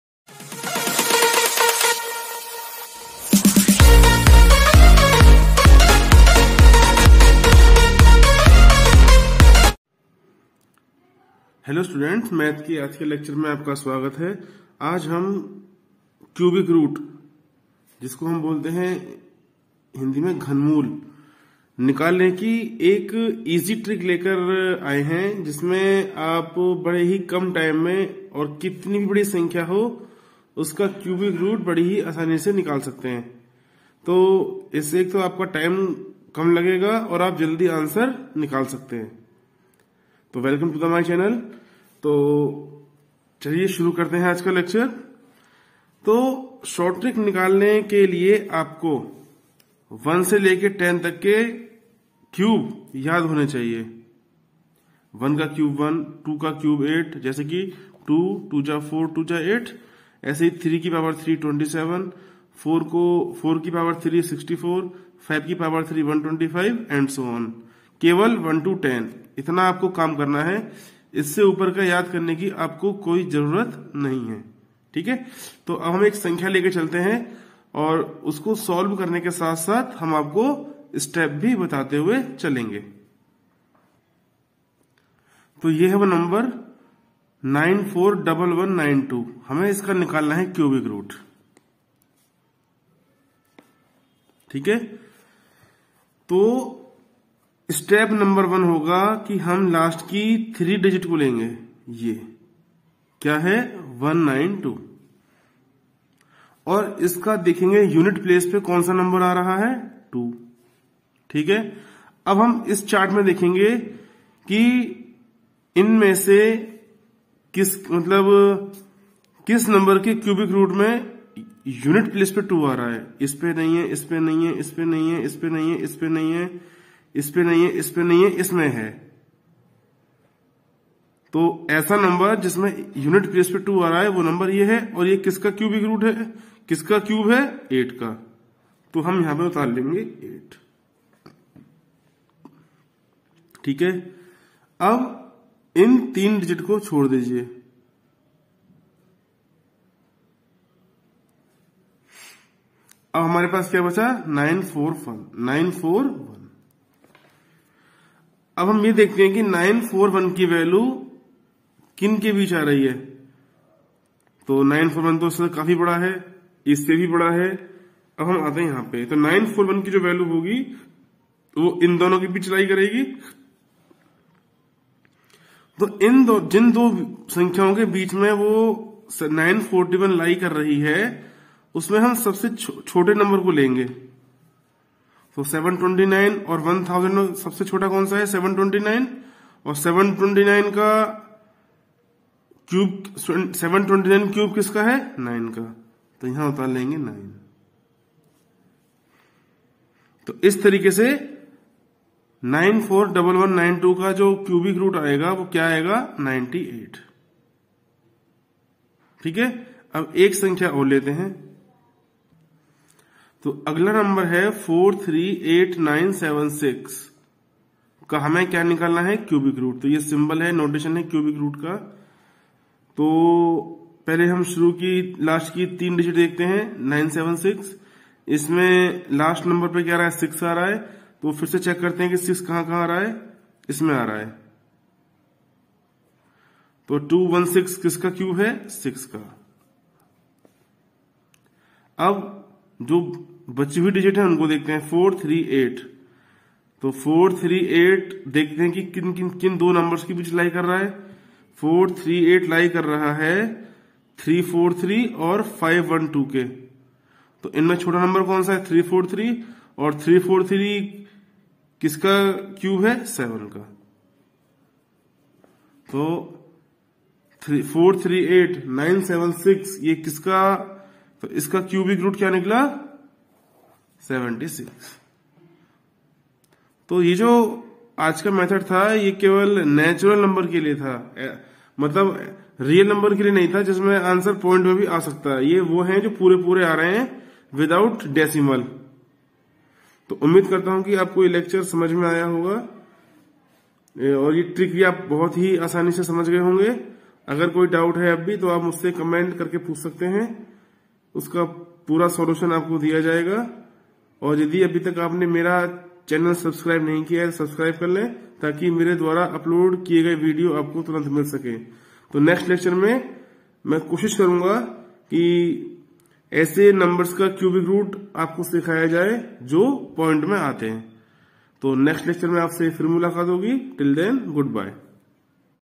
Cubic root निकालने की easy trick
short trick to find cubic root | घनमूल निकालने की short ट्रिक